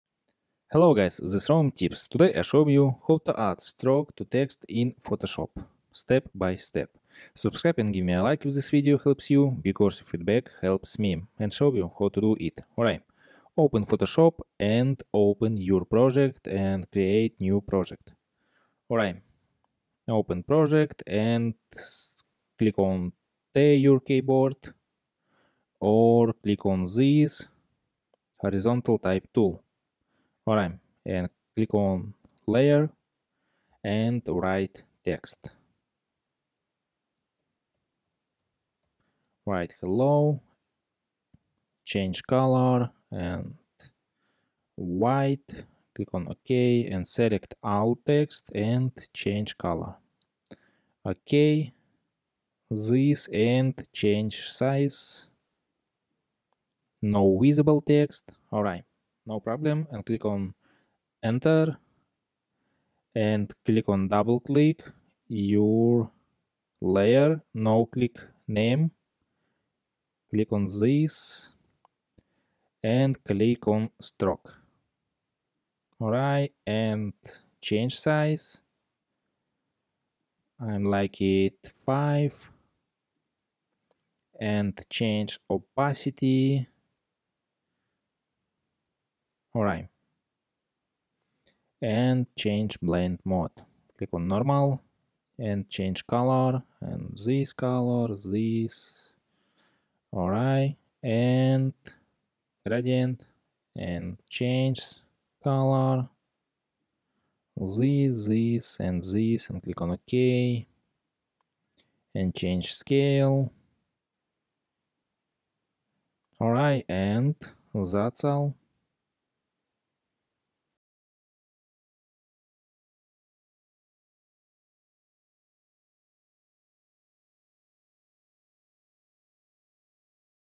How To Add Stroke To Text in Photoshop (Step By Step) | 2024 Complete Guide
Best eSim How To Add Stroke To Text in Photoshop (Step By Step) | 2024 Complete Guide

In this video, I’ll walk you through how to add a stroke to text in Photoshop. Whether you're working on logos or designs, this step-by-step guide will help you outline your text for a bold and clear effect.